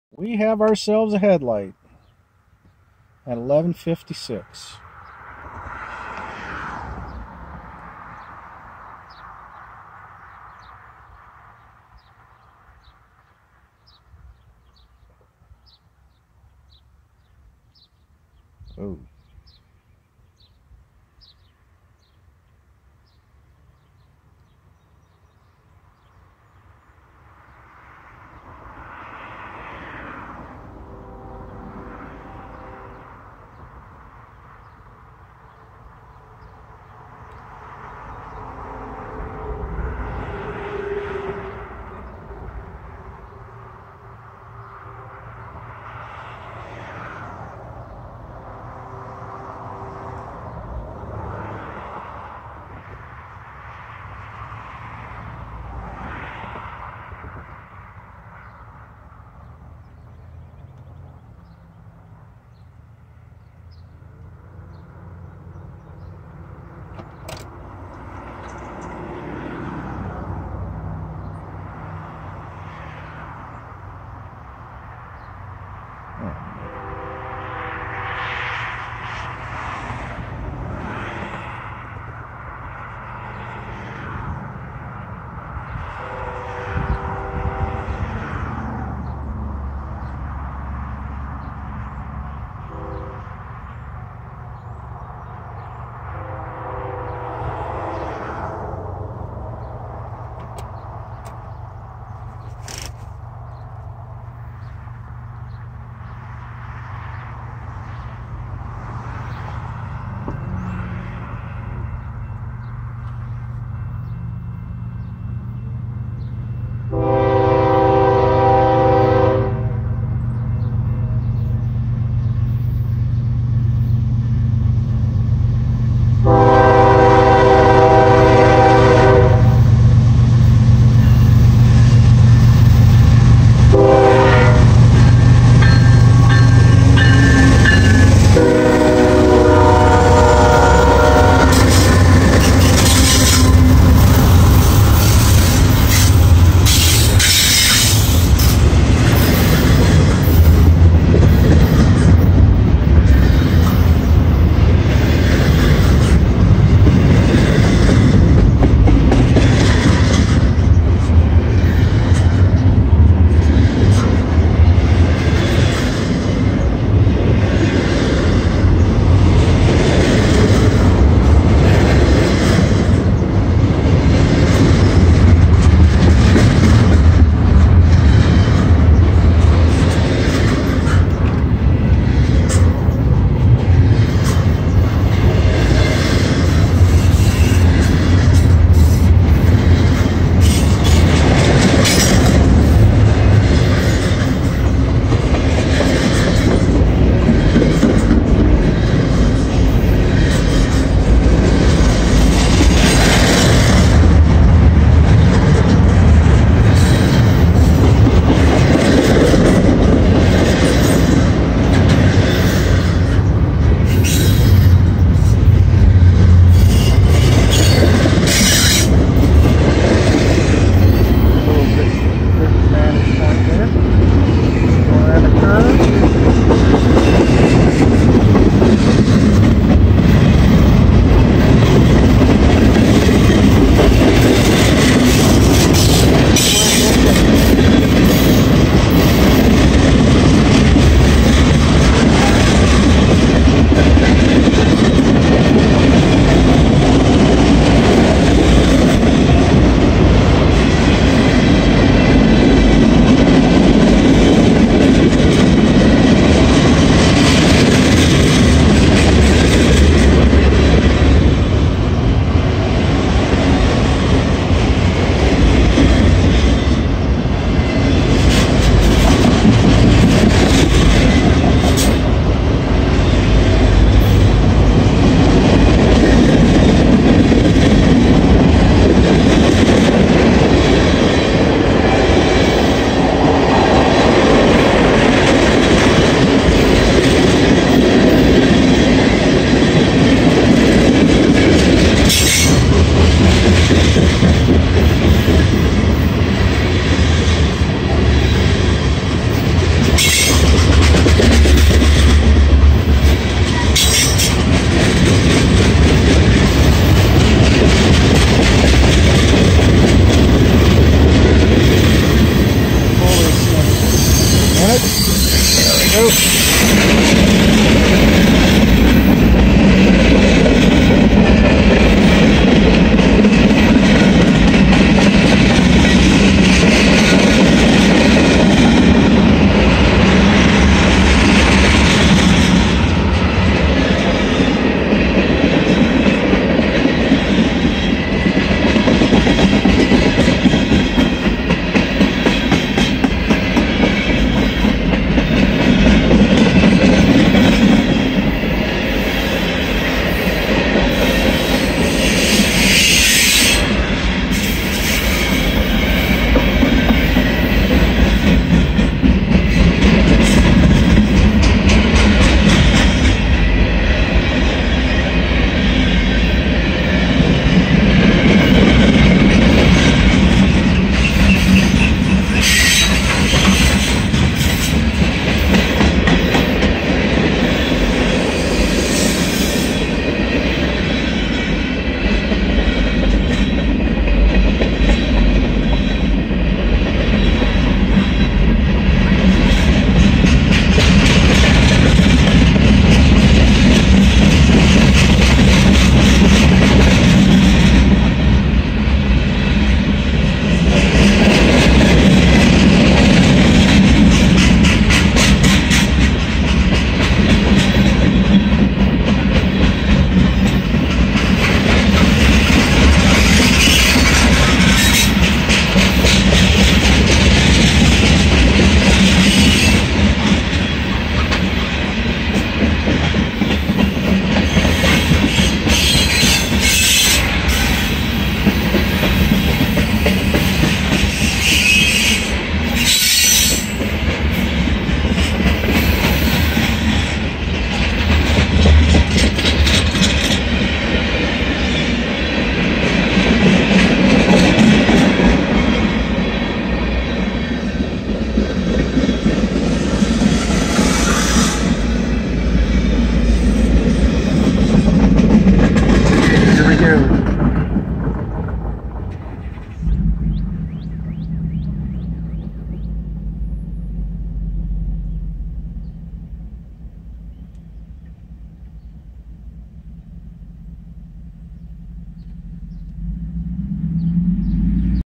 Sitting just south of the Bungalow at US 127 crossing North of town waiting on NS 200 Intermodal
New Miami, Ohio Saturday, April 17th, 2021 about 11:56 a.m. . I was unable to take a video in Glendale. This is actually the second time I have seen this train but the first time I've been able to put anything on my YouTube channel in regard to him. That's not exactly true because I just uploaded the second video north of Collinsville. Had to wait because the smile was so large because there was the anticipation and recording of him going through four grade crossings.